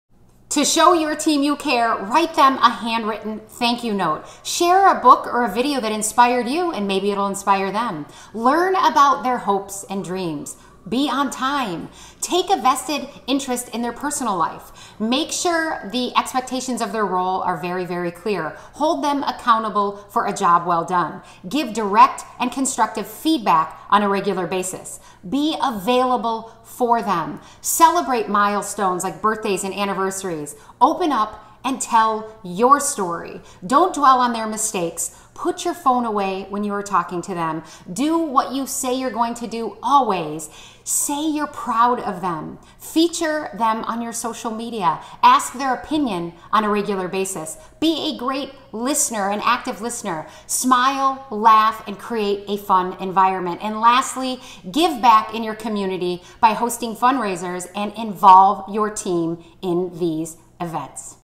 20 Ways To Create A Positive Working Culture | Employee Recognition Ideas
⇒ Create a positive working culture for your team!

In this video, I share 20 ways you can create a positive working culture and keep your team motivated. These employee recognition ideas build appreciation and rapport with your team, which increases trust. Your team should feel they matter, and that should be reflected in the way you treat them daily.

Treating your team in a way that’s professional but amicable is a great way to build rapport, trust, and confidence. Use these ideas to make each member of your team feel valued and appreciated.

Treating team members with an emphasis on their personal qualities and interests fosters deeper relationships with lasting bonds. Being thoughtful to your team, sending appreciation messages, remembering birthdays, mentioning them on social media, trying to actively be a part of their lives fosters greater engagement and inspiration and create a positive working culture.

It is important for team leaders and employers to embody their highest values when engaging with their team. Treating team members like they’re disposable is unproductive; a team leader that engages in a human, empathetic manner will maximize engagement, motivation, and trust. These employee recognition ideas will lead to better results, and an all-around better working environment, improved mood, communication, rapport, and better workflow.